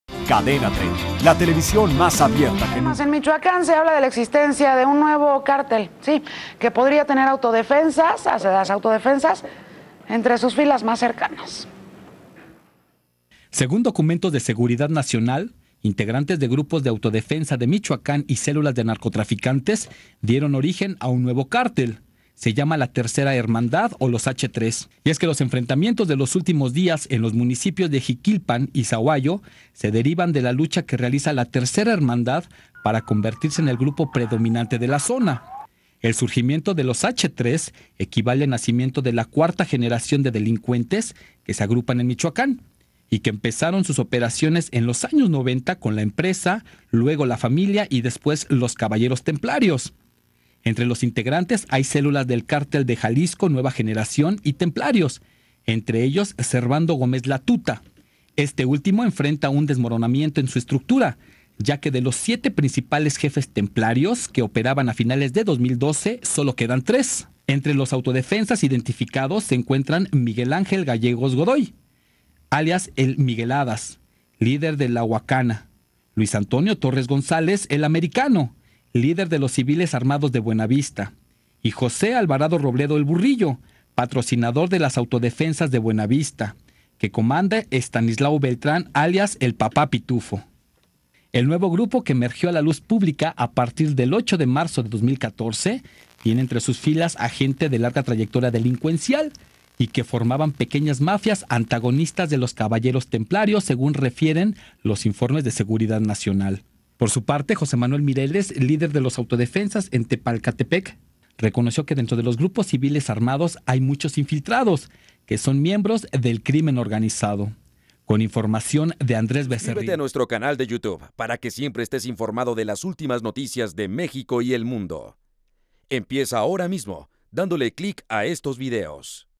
Nace nuevo cártel "Los H3" en Michoacán / Cártel la Tercera Hermandad
06 mayo 2014

En Michoacán nace un nuevo cartel llamada "Los H3", el cual puede tener autodefensas.